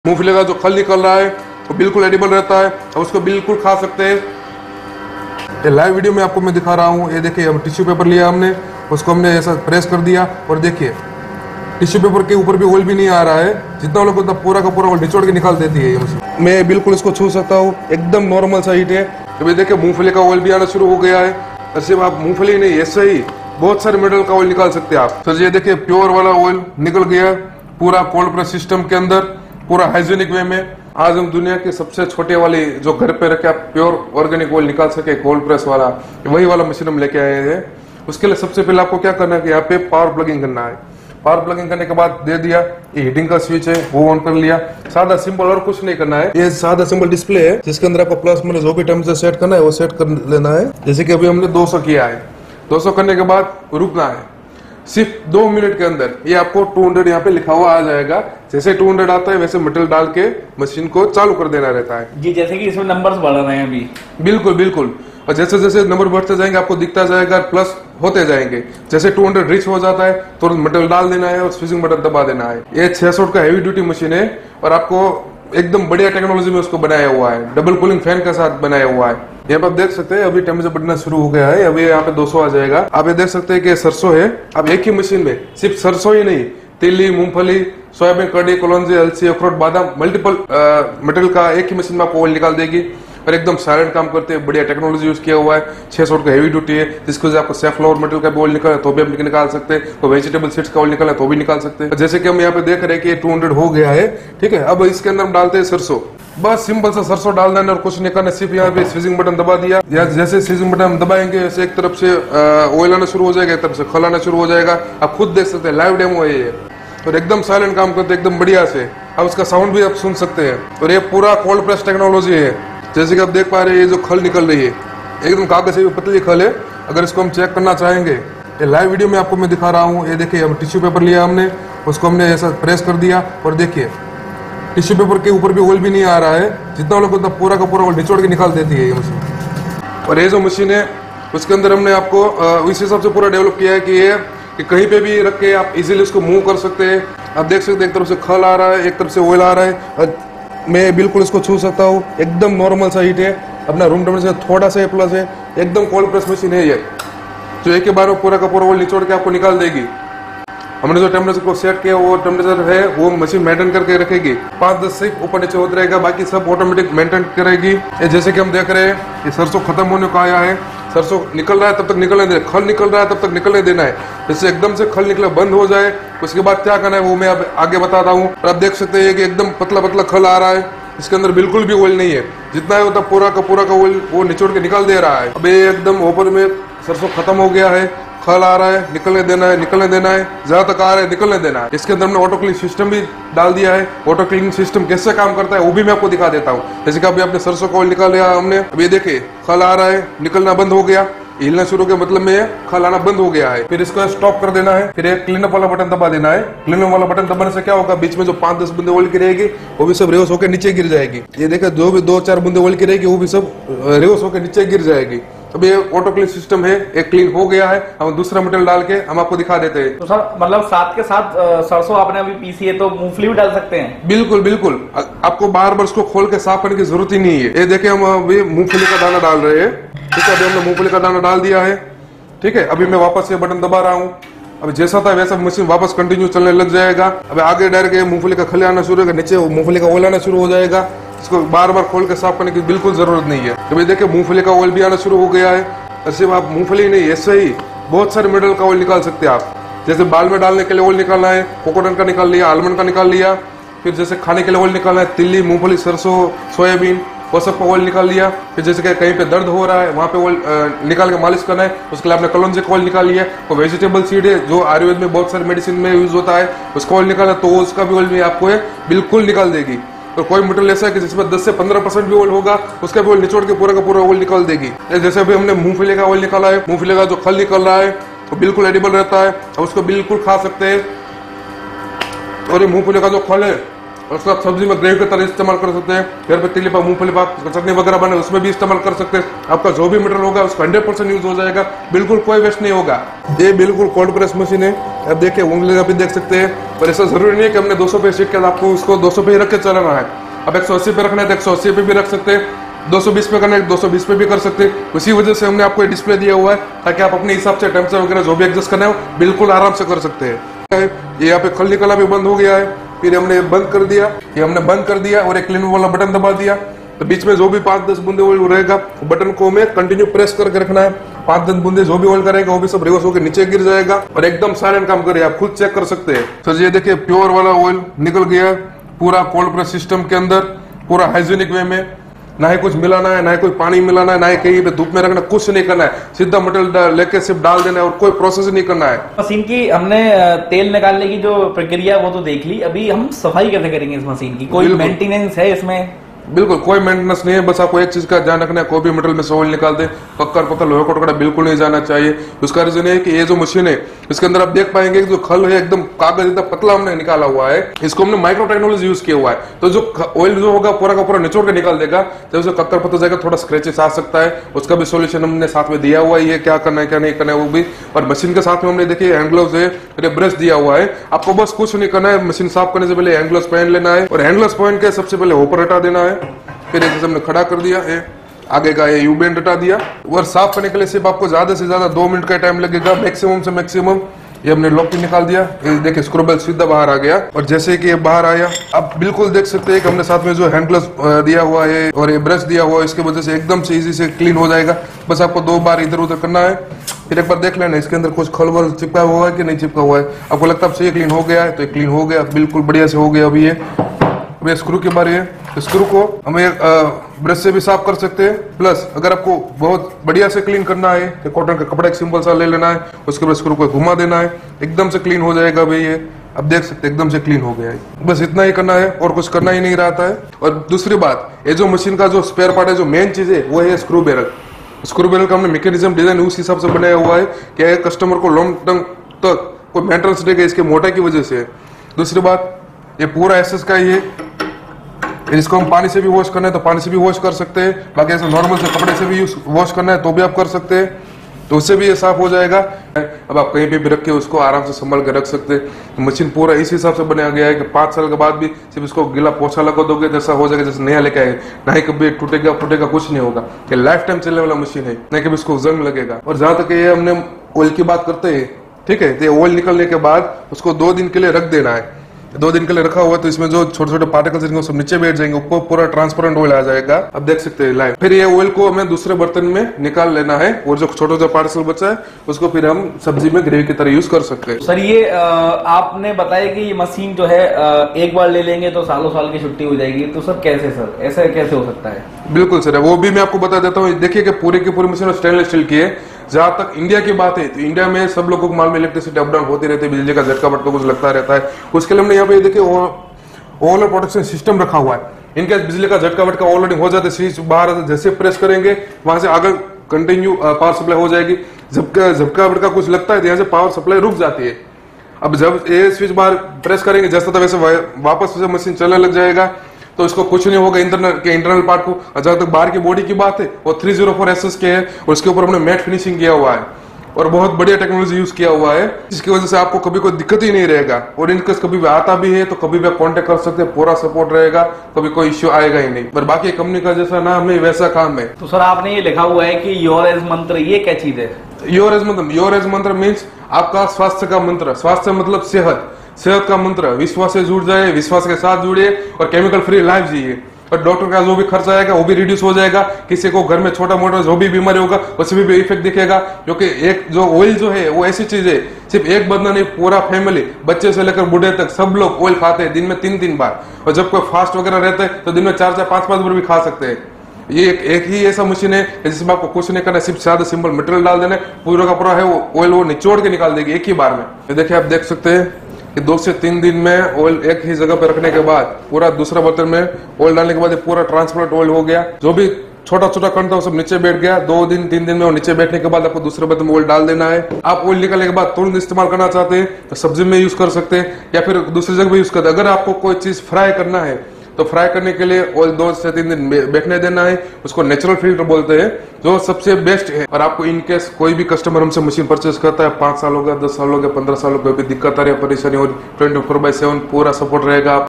Vivatra Vishvas Oil Maker Machine For Home | Cold Oil Press Machine | Oil making Machine
Vivatra Vishvas Oil Maker Machine For Home Use Vi-582Tc 600 Watts Cold Oil Press Machine With Multiple Seed Simplified Digital Temperature Controller, Stainless Steel